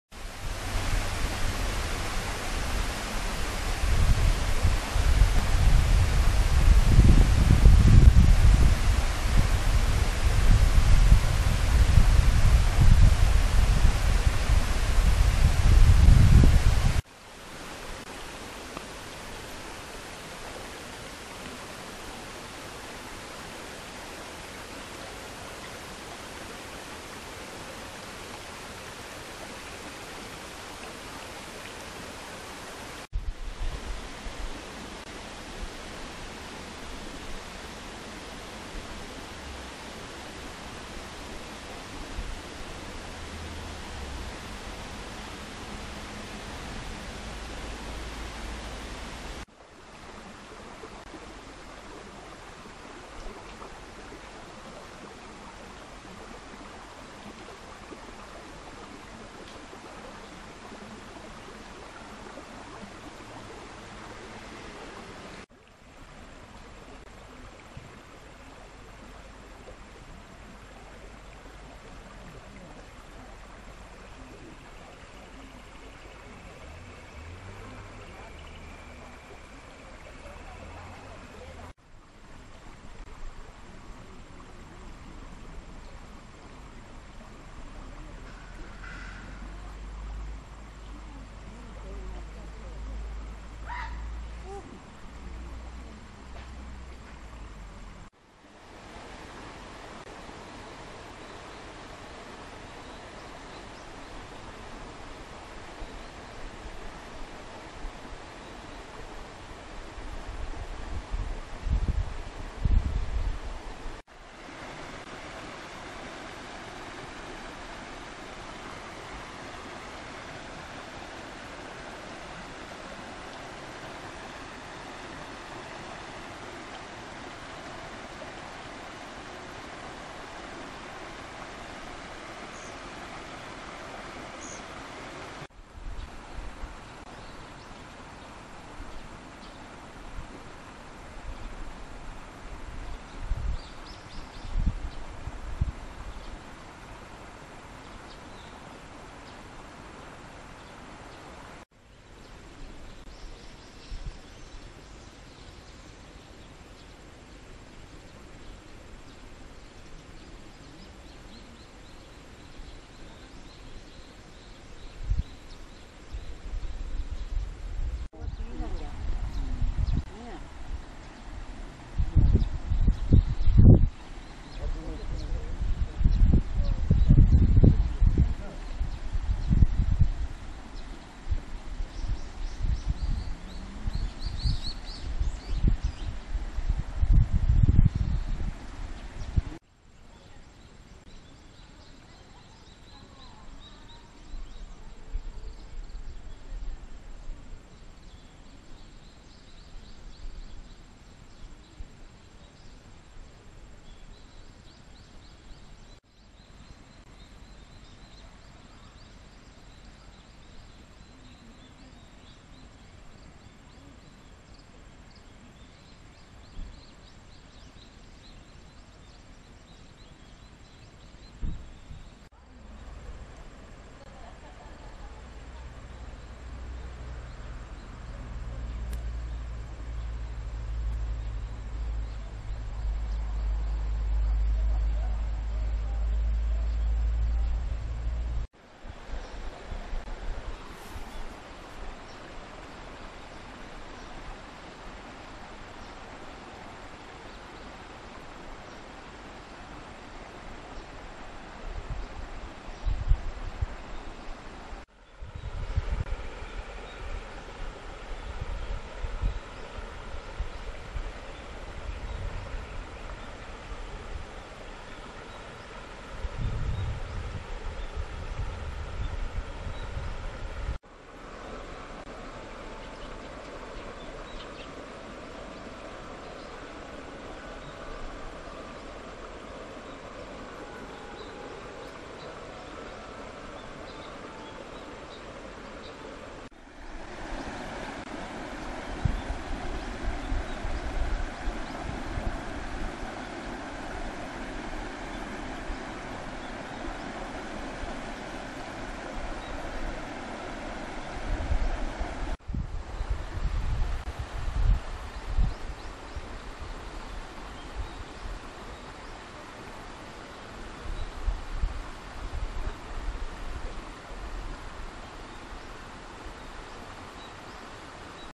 三滝堂4K DEMO HERO3 ブラックエディション
Gopro　HERO3　ブラックエディションの4K動画を登米市の名所三滝堂で撮影しました。